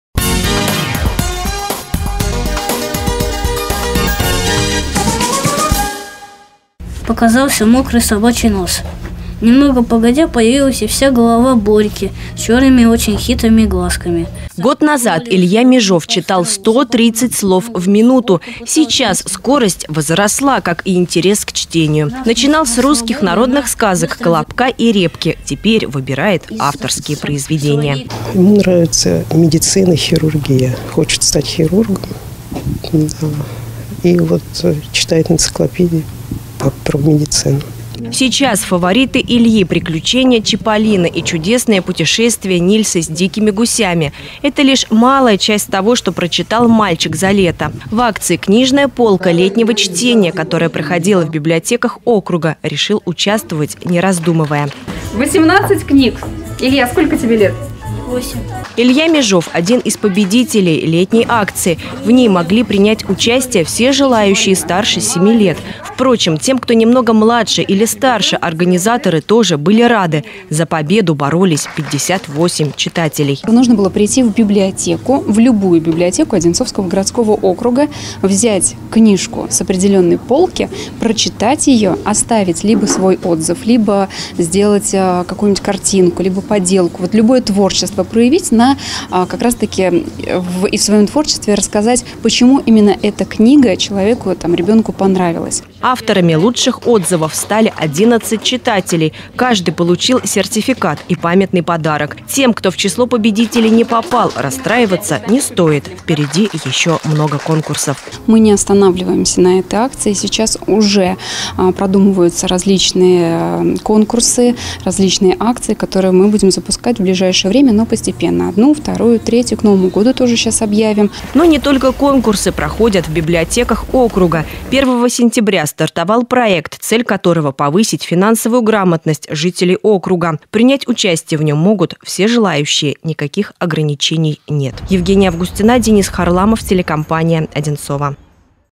Книжная полка летнего чтения
Итоги акции «Книжная полка летнего чтения» подвели в Одинцовской библиотеке №1. Победители получили сертификаты и памятные подарки.